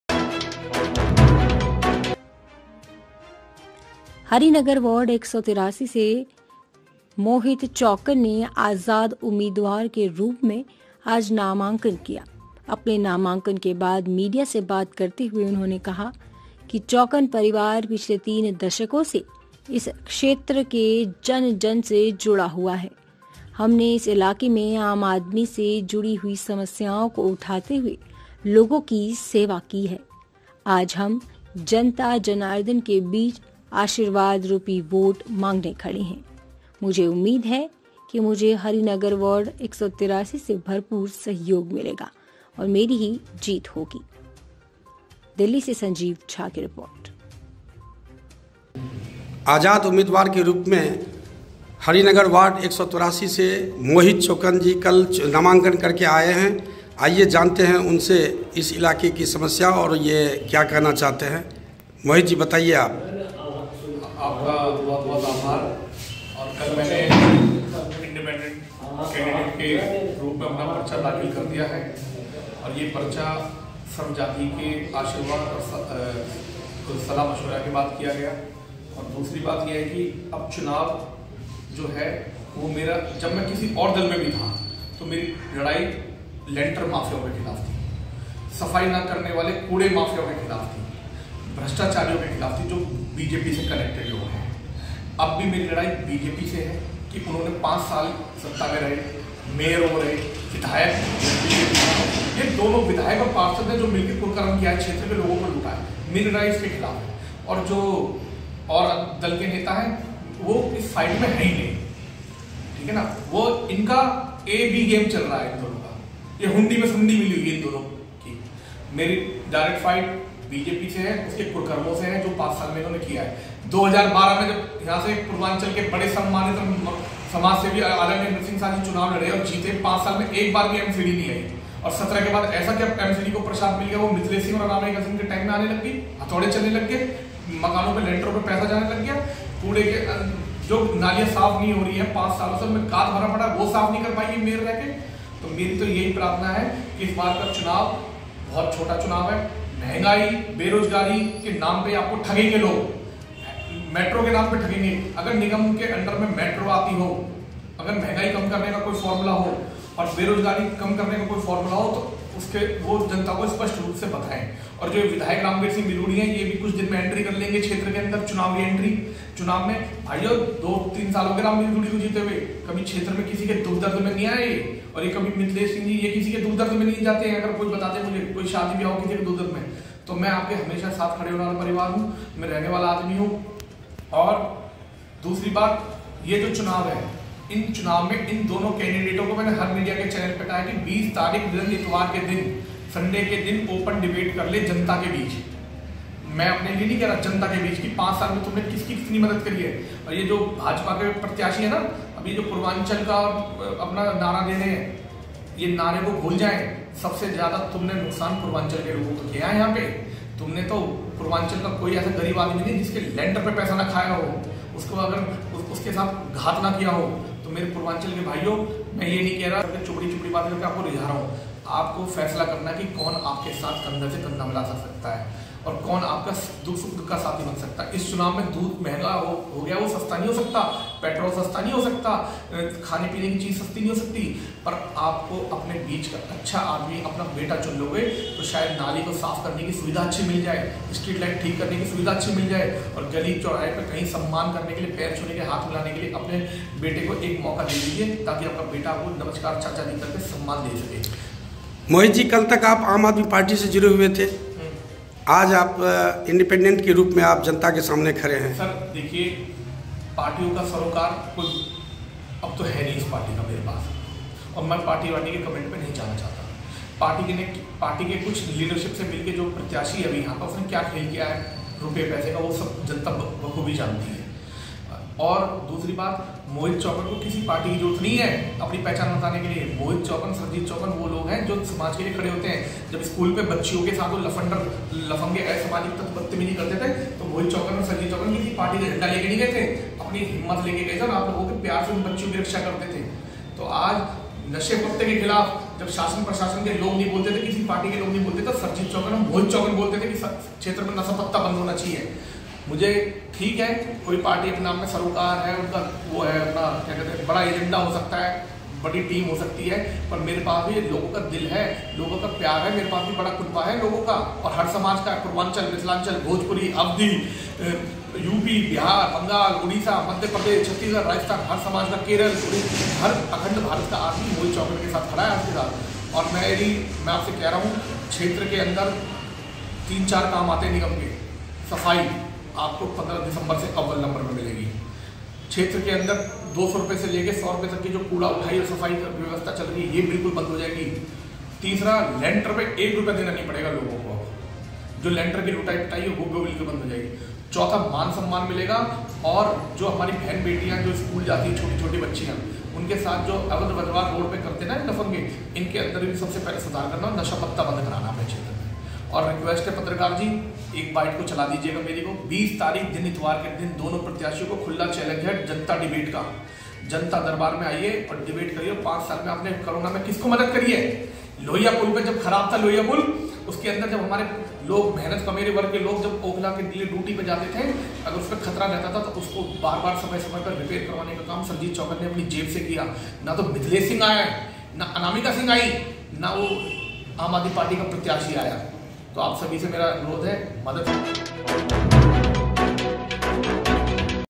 नई दिल्ली : हरी नगर वार्ड 183 से मोहित चौकन ने किया नमांकन।
स्लग- हरी नगर वार्ड 183 से मोहित चौकन ने किया नमांकन।
संजीव झा
नई दिल्ली, हरि नगर वार्ड 183 से जनता के सेवक मोहित चौकन ने आजाद उम्मीदवार के रूप में आज नामांकन किया। अपने नामांकन के बाद मीडिया से बातचीत करते हुए उन्होंने कहा कि चौकन परिवार पिछले 3 दशकों से इस क्षेत्र के जन-जन से जुड़ा हुआ है। हमने इस इलाके में आम आदमी से जुड़ी हुई समस्याओं को उठाते हुए लोगों की सेवा की है।आज हम जनता जनार्दन के बीच आशीर्वाद रूपी वोट मांगने खड़े हैं। मुझे उम्मीद है कि मुझे हरि नगर वार्ड 183 से भरपूर सहयोग मिलेगा और मेरी जीत होगी। उन्होंने कहा कि आम आदमी पार्टी ने उनके साथ नाइंसाफी किया जिससे पार्टी के वर्करों में भी भारी आक्रोश व्याप्त है।वह जनता के फैसले का सम्मान करते हुए इस वार्ड से नामांकन किया। चौकन ने कहा कि आज यह क्षेत्र विकास से कोसों दूर है। नहर पार इलाके में ओ-जोन की समस्या एक आफत बनी हुई है। हम इस मुद्दे पर पिछले एक दशक से ज्यादा समय से सरकार से इलाके की इस भीषण समस्याओं से निजात दिलाने की अपील करते रहे किंतु आज भी यह समस्याएं जस की तस बनी हुई है। क्षेत्र के लोगों के साथ वह आज से नहीं दशकों से जुड़े हुए  हैं। आज पूरी दिल्ली में मेट्रो का जाल बिछ गया किंतु यह इलाका आज भी यात्री सुविधा के नाम पर मरहूम है। आखिर यहां के लोगों का क्या कसूर है?